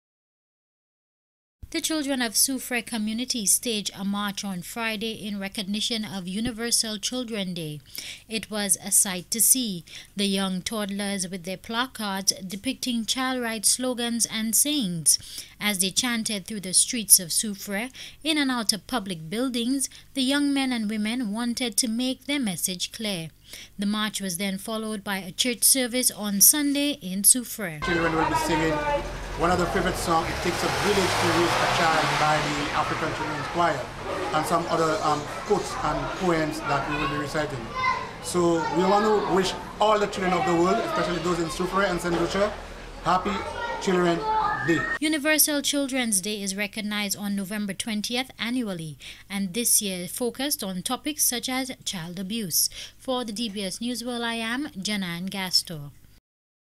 A toddler's Voice
Several pre-schoolers from Soufriere took to the streets on Friday to help raise awareness of child rights issues…more specifically awareness of child abuse. Organizers believe public awareness can be an important part of an overall approach to preventing child abuse and neglect. Jenna Anne Gaston explains